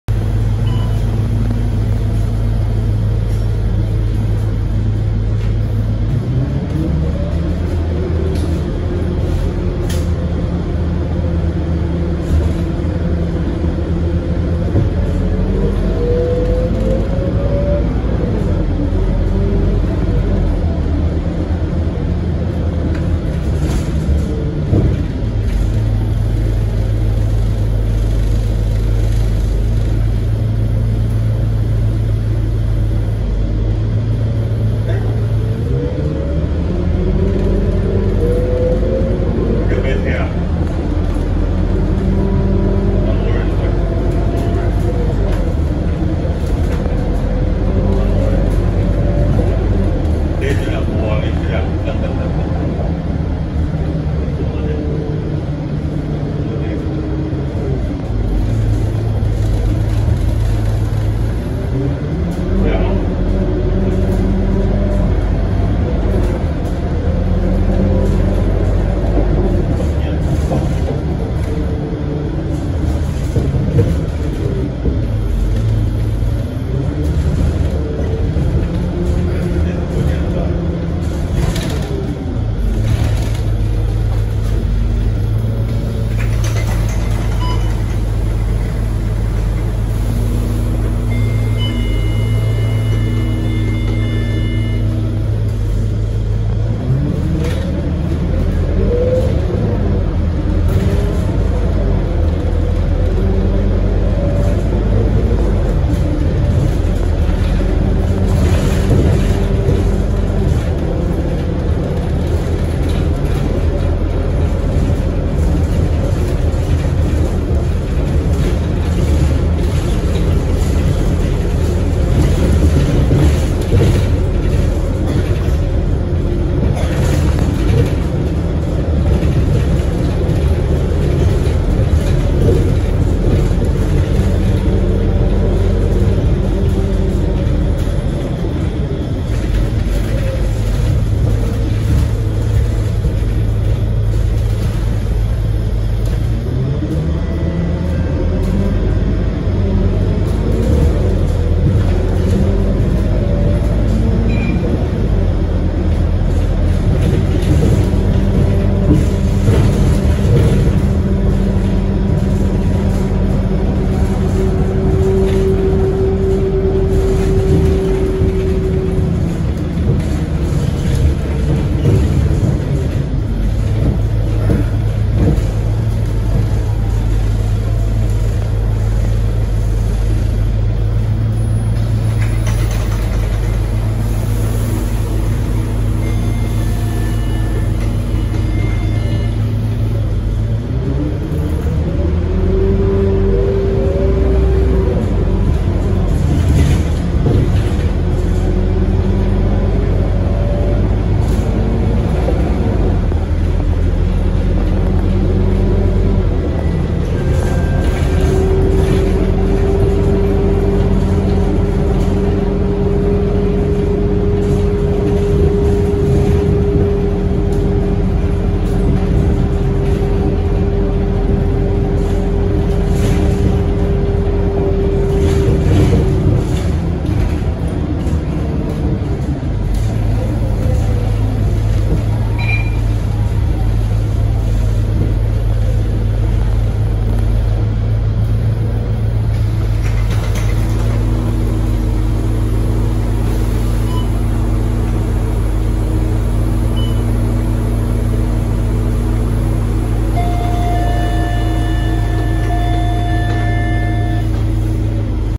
SBS Transit Scania K230UB (Euro 5 Batch 1) SBS8726M on Service 63
Recorded on 31 July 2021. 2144hrs-2148hrs. Just film a short Scania K230UB (Euro 5) single decker on SBS Transit Service 63 from Macpherson to somewhere at Geylang East. Basically, I don't really want to film for long.

Anyway, I intended to do Exit door opening & closing of Scania K230UB (Euro 5 Batch 1), but I decided to do other time when I board SBS Transit Service 63 again. It's better I took Euro 5 these days, my favourite variant of Scania K230UB!

Published on 18 August 2021, 1400hrs

Specifications:
Journey Rating: 94%
Start route: Block 79 FC (70211)
End route: Block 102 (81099)
Stop recording: Aljunied Road